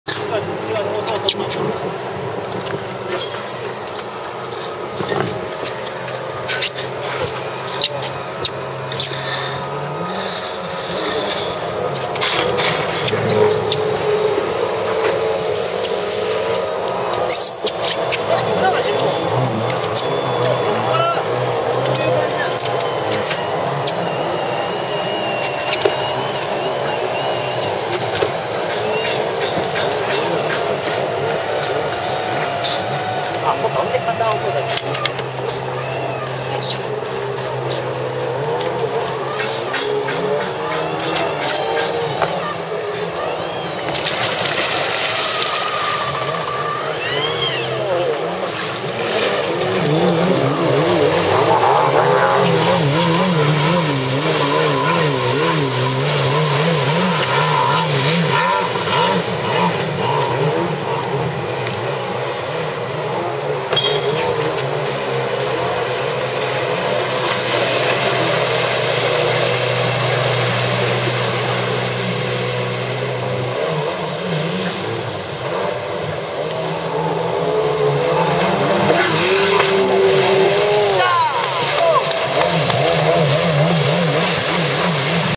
Freestyle Jet Ski Chine " Ryu ken yici "
Jet Ski Yamahe Standard !
Vrille avec moteur Origine

Very Good